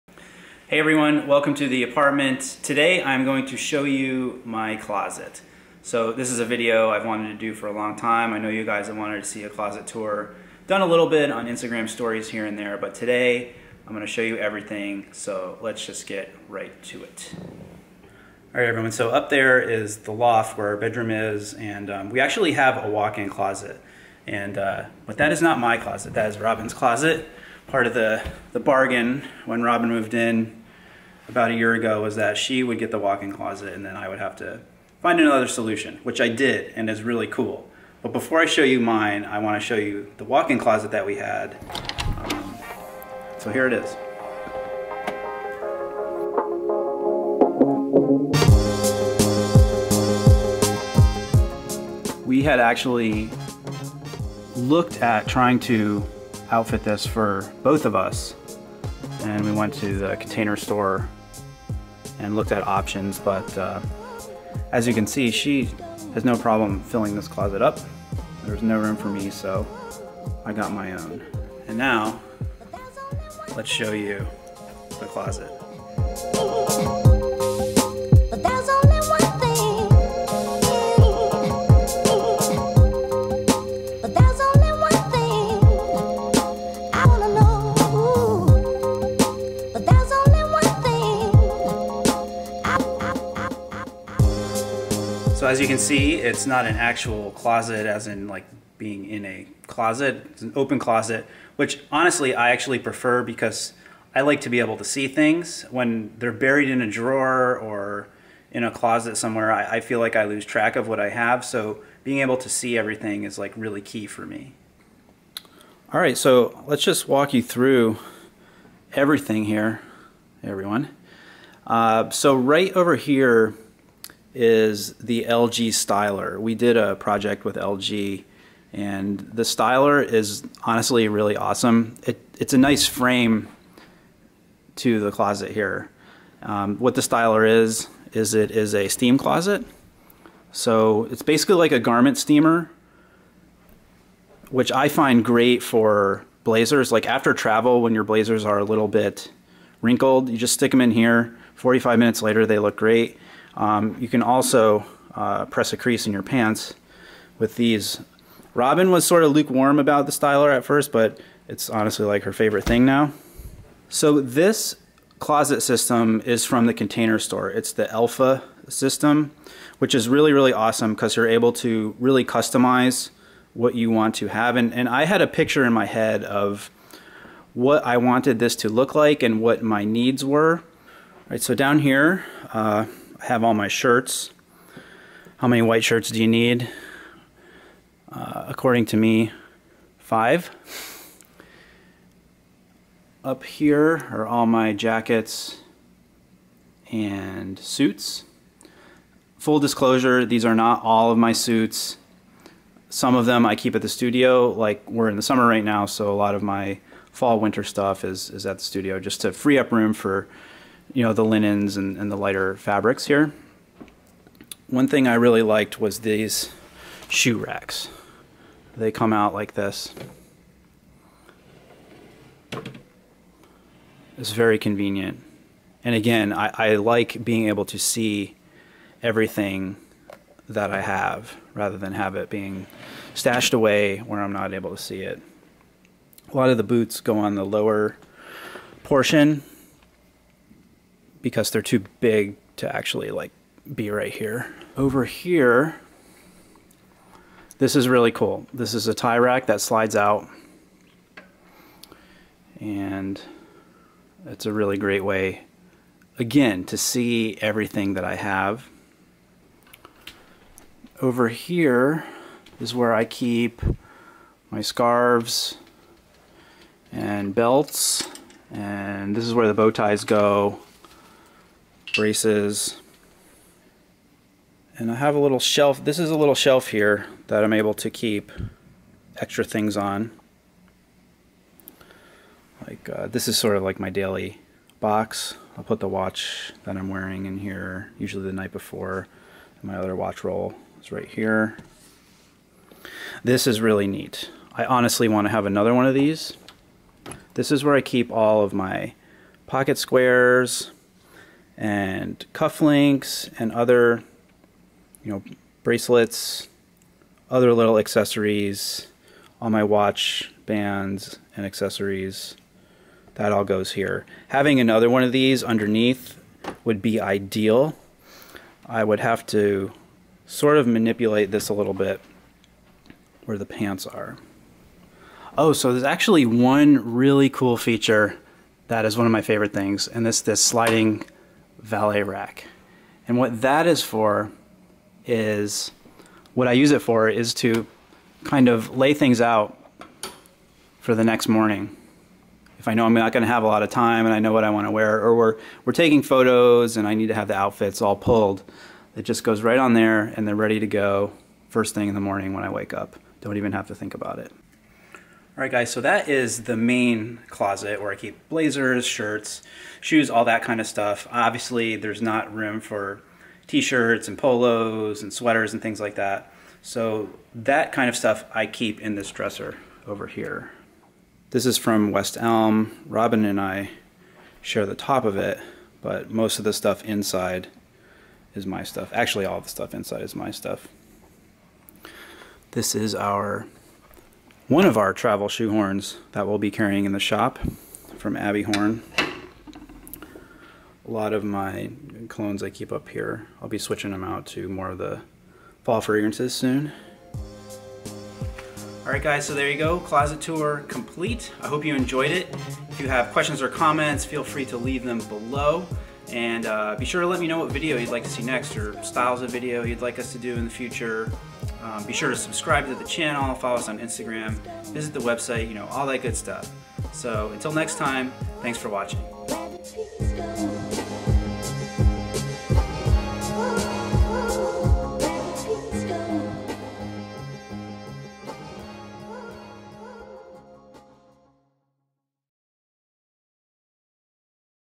Check Out My Closet! | HSS Closet Tour
In today's video I'm giving you a tour of my closet! Frequently requested, I'm excited to finally give you a look into my closet and how I keep everything organized.

We live in a open-floor plan loft apartment, so that means that closet space is more or less limited. There is a walk-in closet in the apartment, but when you've got a stylish SO, that means there's not a whole lot of room in that closet.

Though we did try to figure out a way to outfit the walk-in closet to suit both of our needs, we finally determined that it would simply be too cramped, so I handed over the walk-in closet to Robin and embarked on finding my own solution.

The system I decided on after a lot of thought, design and comparison is from the Container Store - their Elfa system. Highly customizable, practical and, of course very stylish. Checks all the boxes! I'm pretty pleased with how it's configured and how it came out.

I realize that I left out a lot of details in terms of the specific brands of some of my clothing, shoes and accessories - didn't want this video to get super, super long. But, if you're interested in knowing more, I can definitely shoot a follow-up to get more detailed.